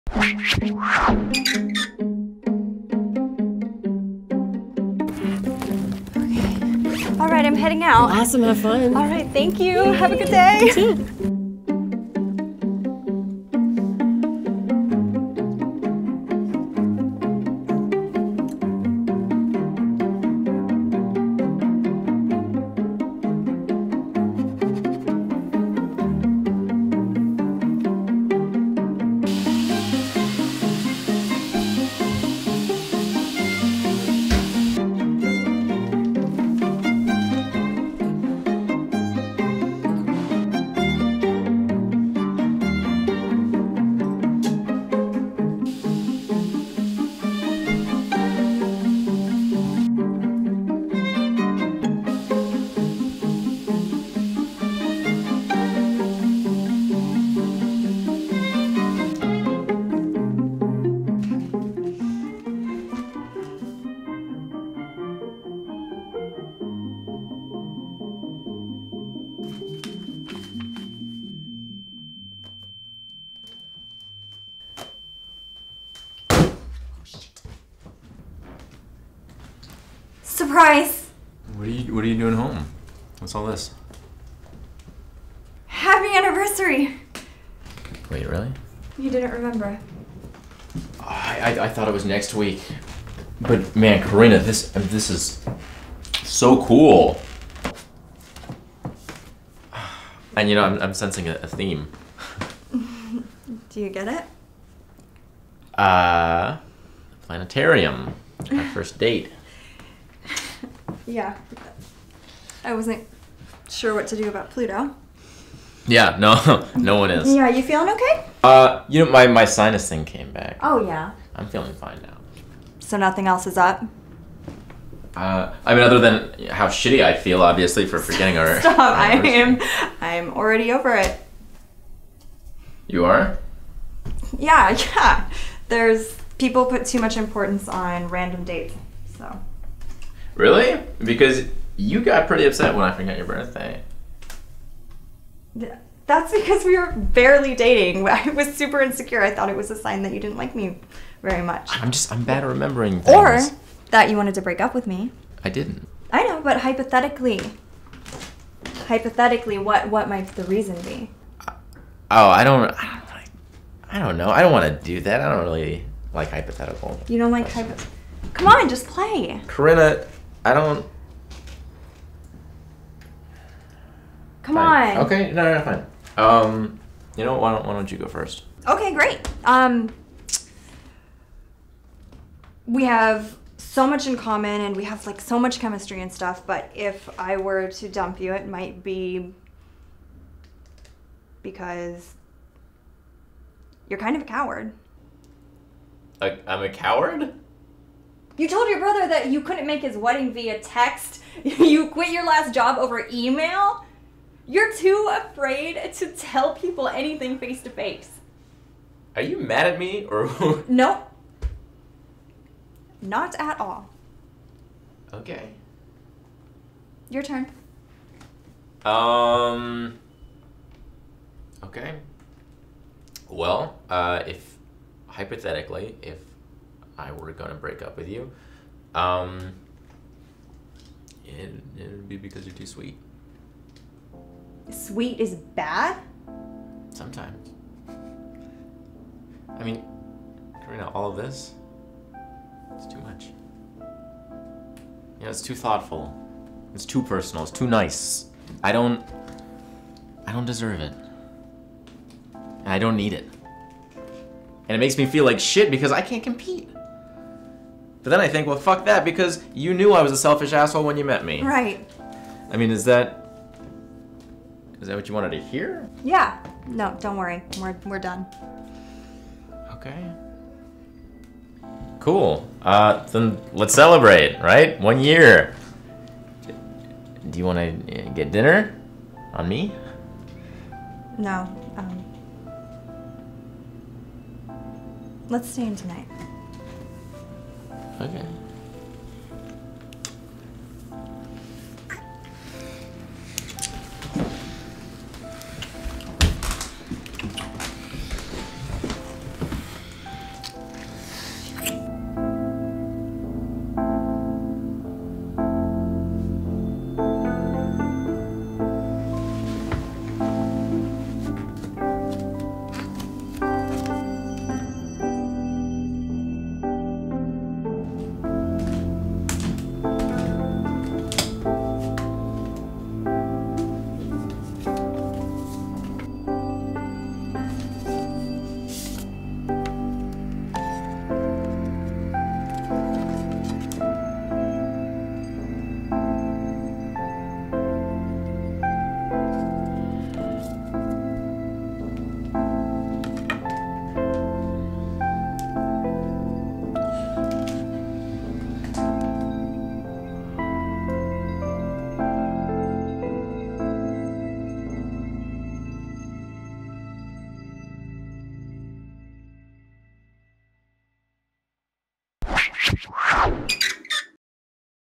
Breakup Sex
Sometimes the best way to end things is with a bang.

MUSIC
Absent Friends

FEATURING
Rose Guess
Nick Zephyrin